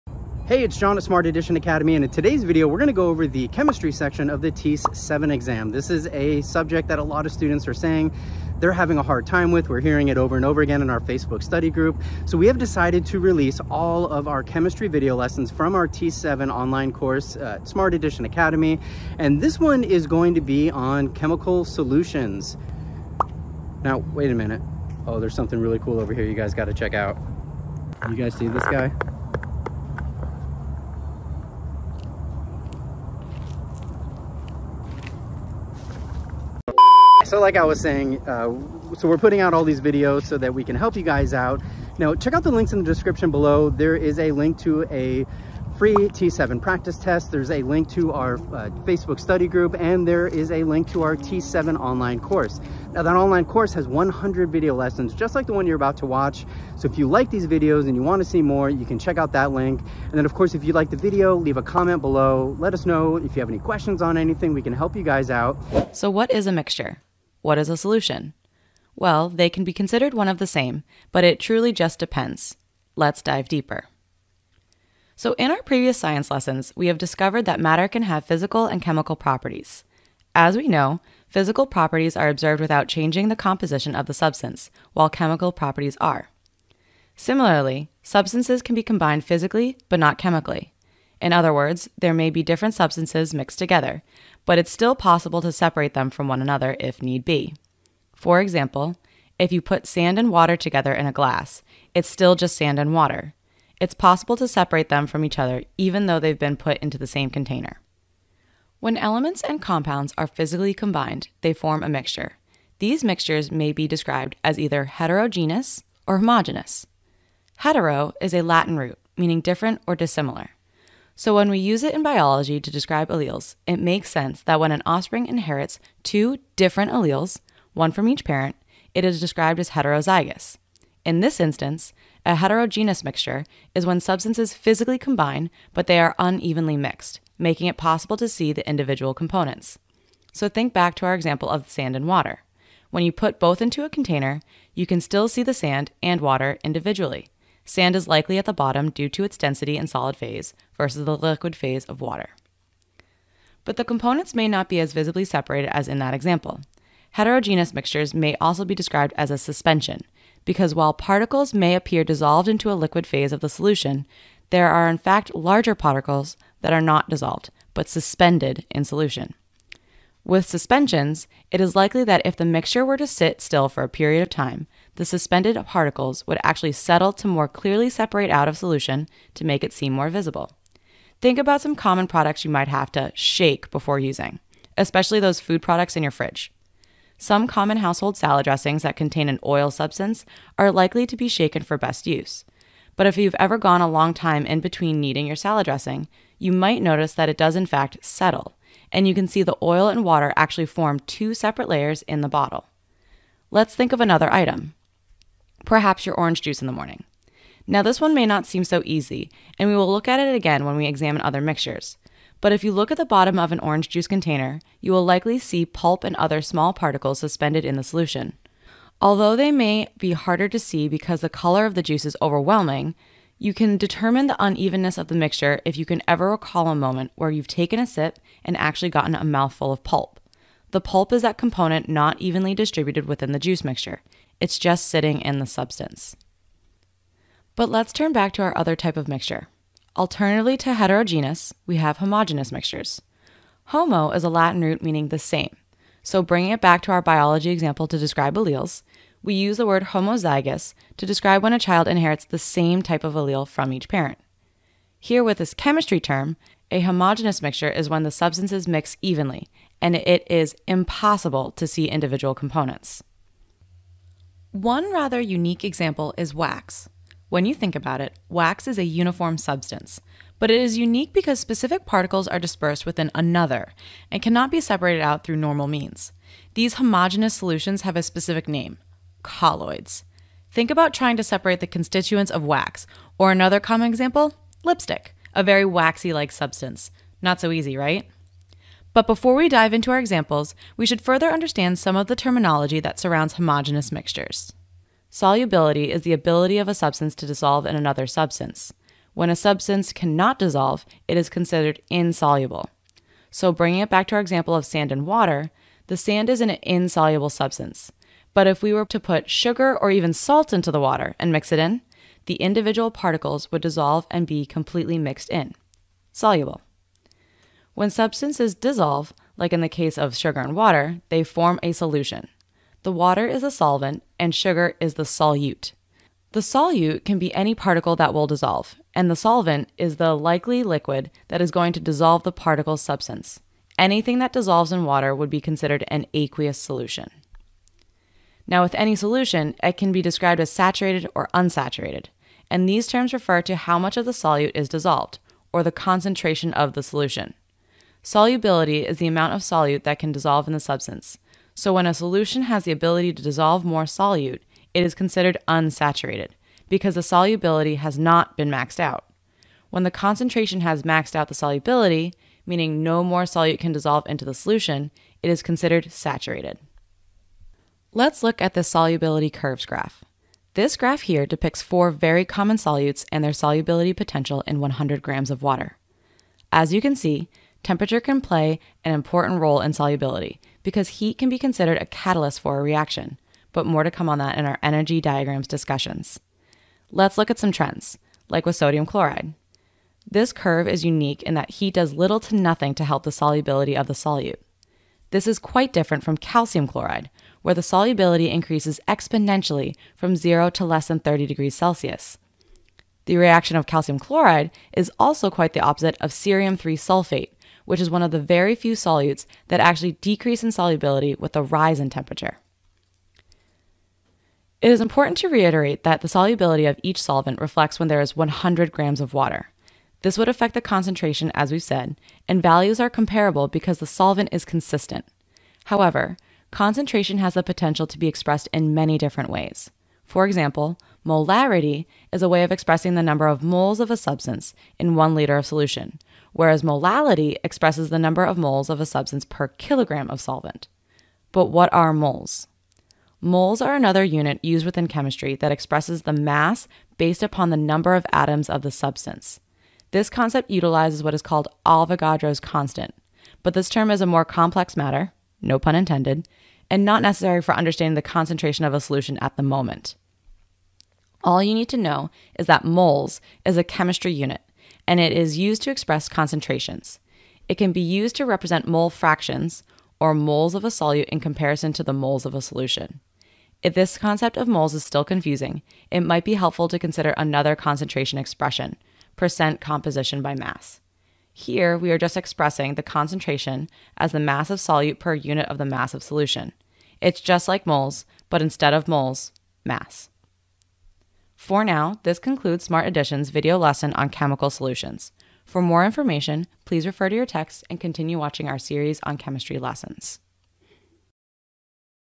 TEAS Chemistry Review | Chemical Solutions
A major part of the TEAS science section is oriented around chemistry. The chemistry concepts test takers are expected to know include chemical bonds, solutions, chemical reactions, acids, bases, scientific notation, and more. This is all in addition to biology, scientific reasoning, and A&P knowledge! While there are only 8 questions on chemistry on the TEAS 7, it's important to answer these questions correctly to earn a great score on your ATI TEAS. As promised in the video, here are some TEAS 7 study resources: